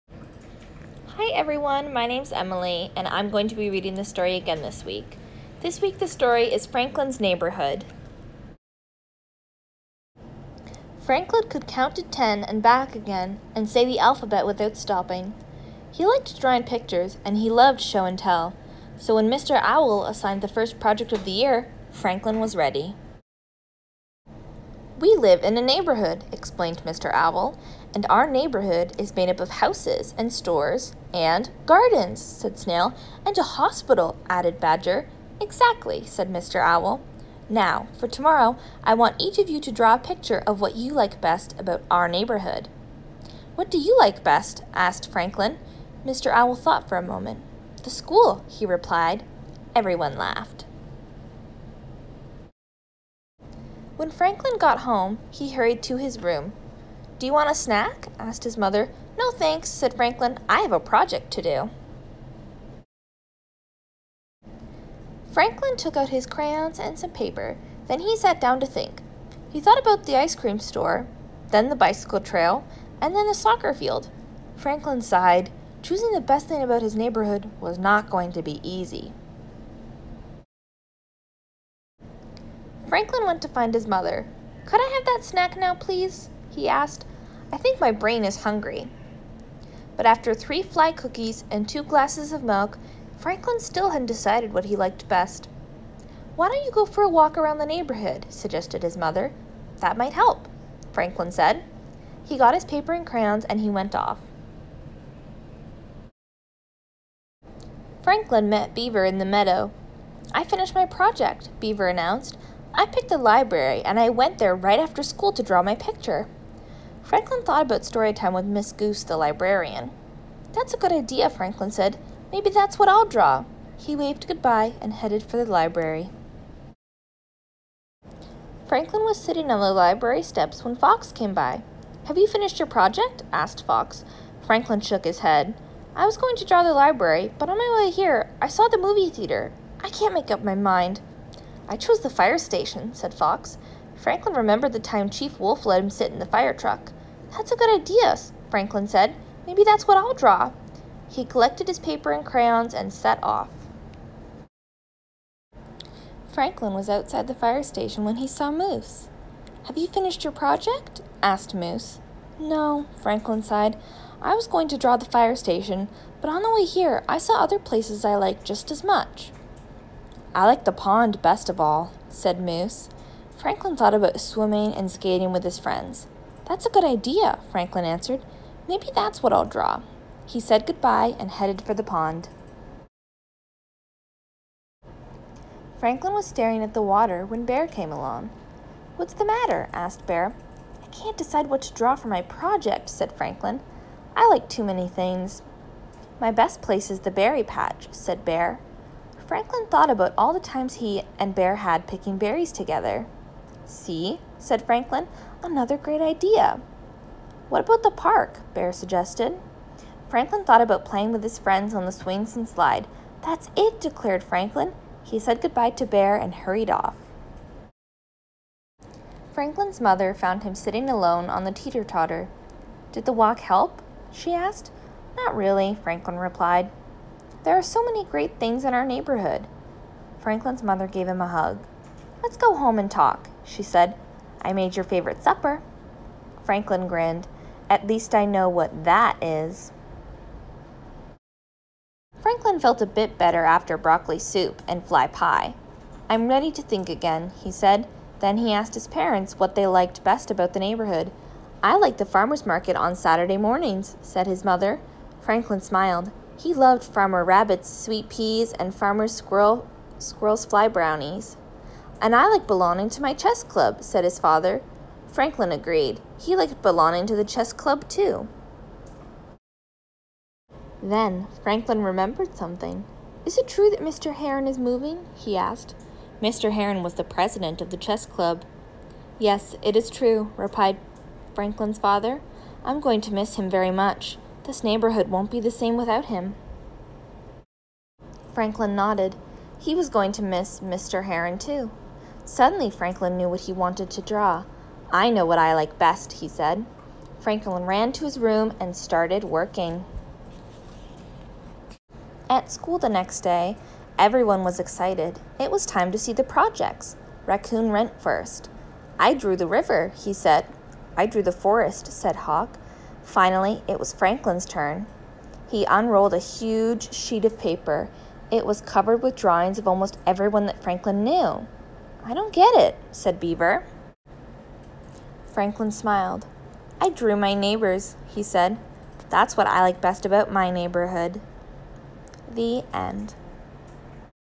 Franklins Neighborhood
Children's Storytime
Written by: Paulette Bourgeois
Illustrated by: Brenda Clark